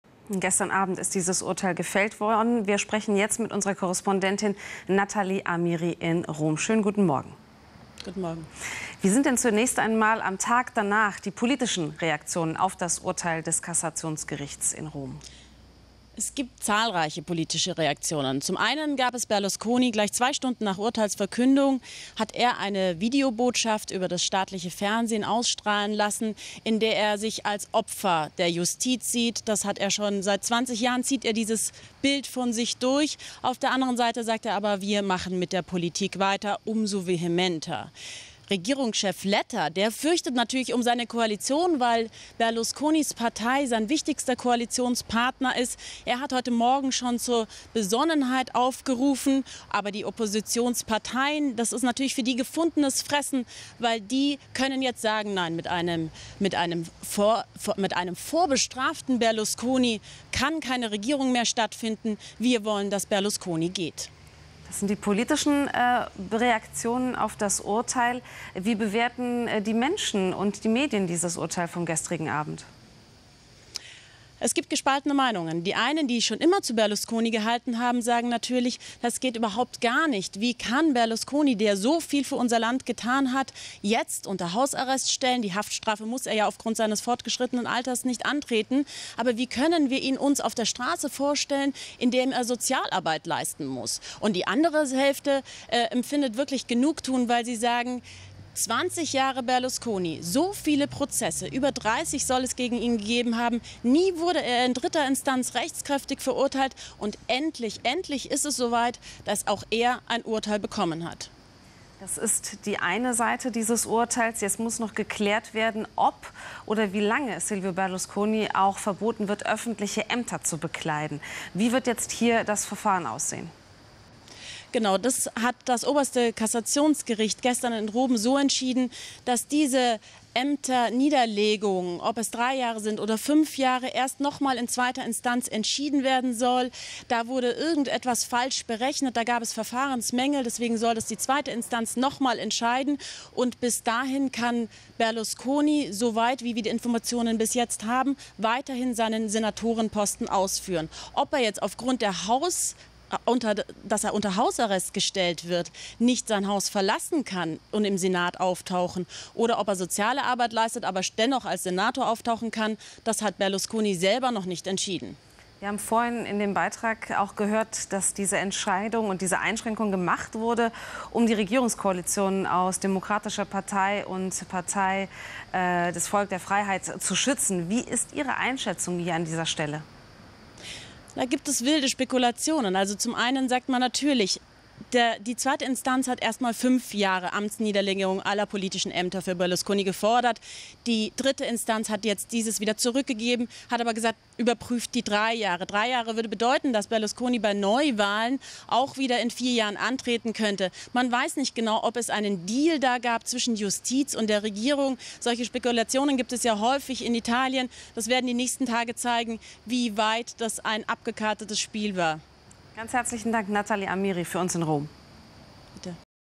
Berlusconi: Interview mit Natalie Amiri am 02.08.2013
Elif Şenel im Gespräch mit Natalie Amiri über die Verurteilung des früheren italienischen Regierungschefs, Silvio Berlusconi